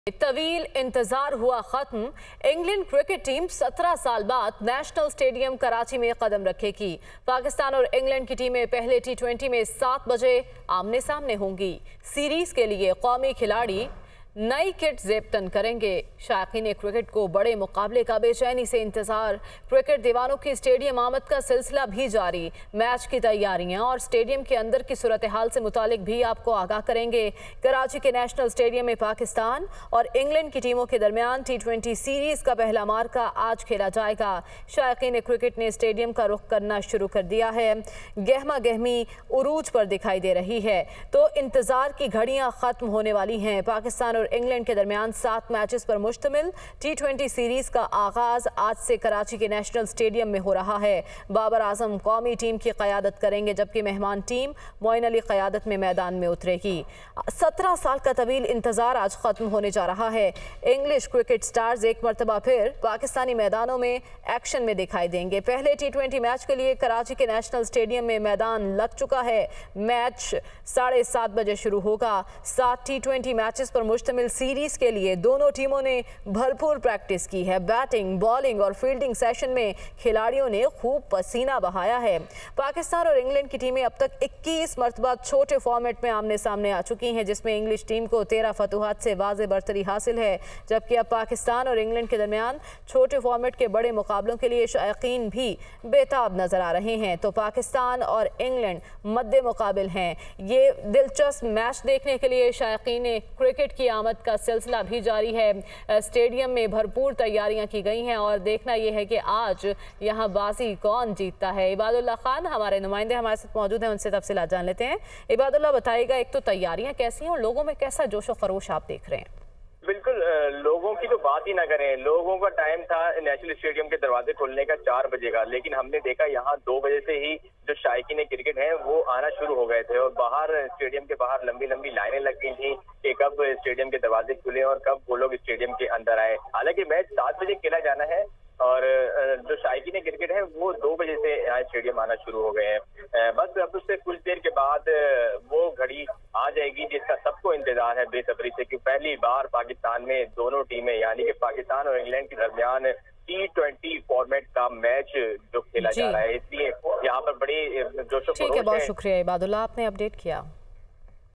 Pakistan vs England 1st T20 Match | Latest Updates
طویل انتظار ختم ،انگلینڈ کرکٹ ٹیم 17سال بعد نیشنل اسٹیڈیم کراچی میں قدم رکھے گی ، شائقین کرکٹ پر جوش